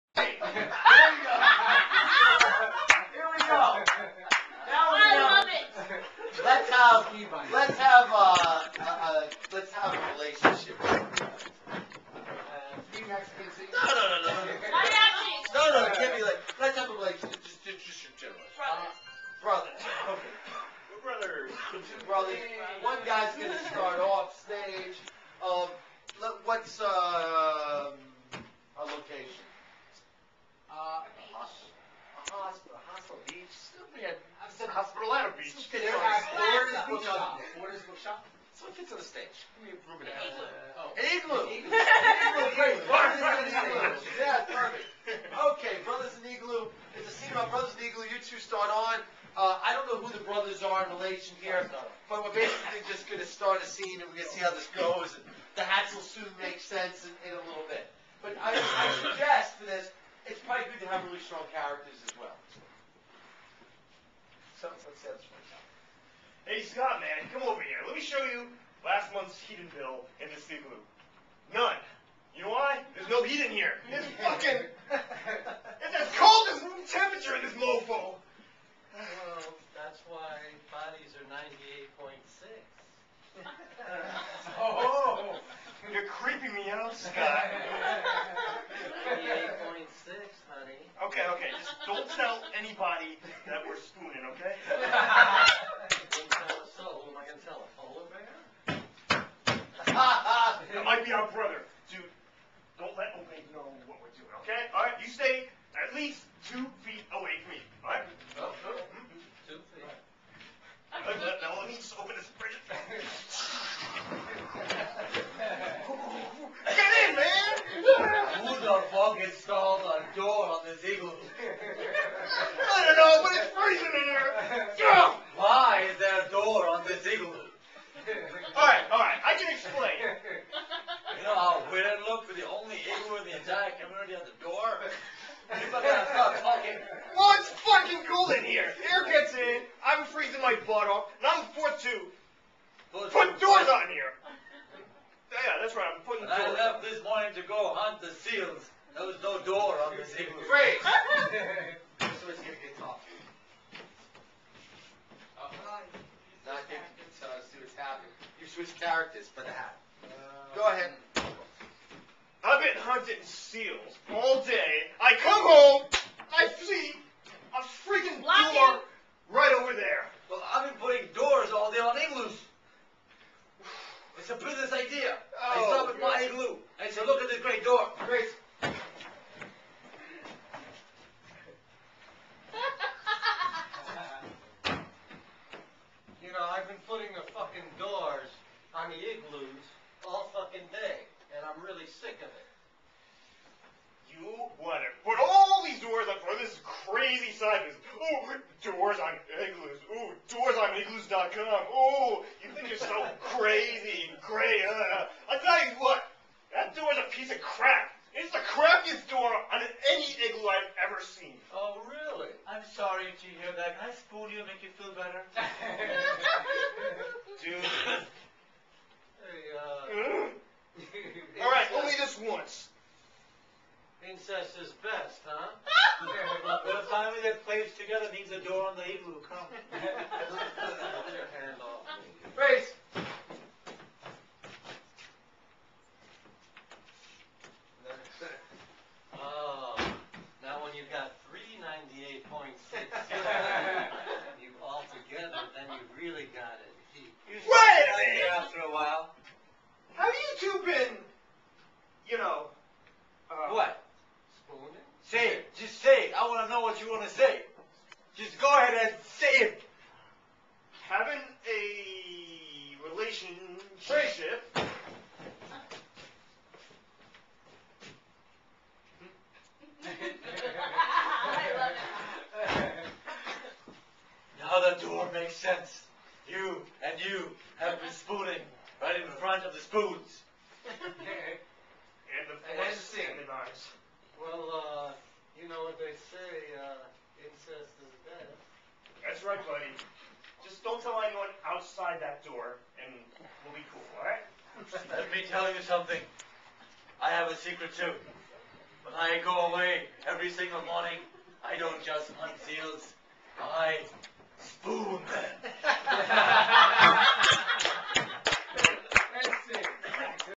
Wed Nite Funny Improv - Hat Switch
As hats are switched so are the roles, creating a challenge for the actors as they overcome a change in point of view.

An igloo with an ugly door sparks controversy between three brothers.